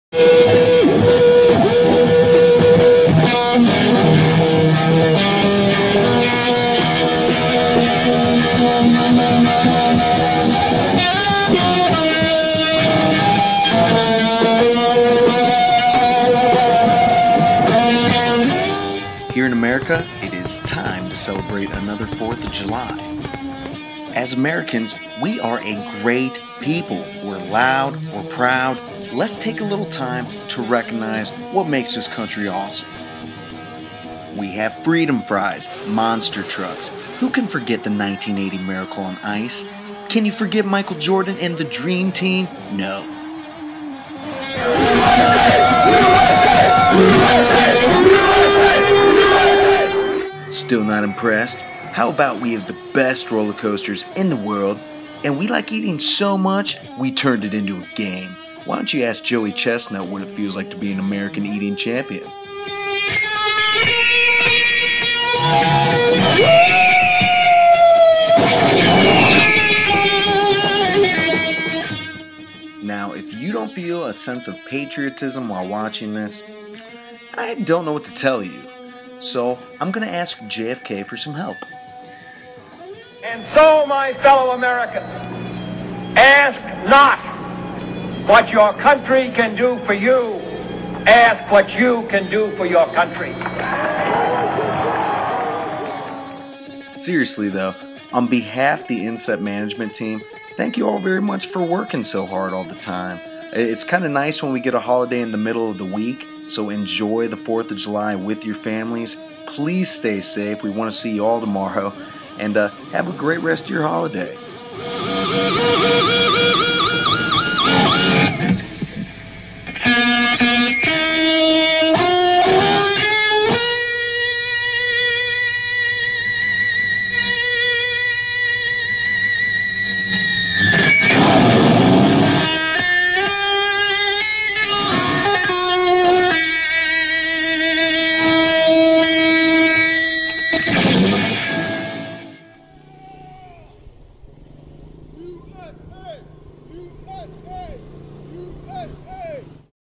Happy Fourth of July From Incept
Here is a very special Independence Day tribute from Incept! Thank you to all of our employees for your hard work and enjoy the day off! Happy Fourth of July!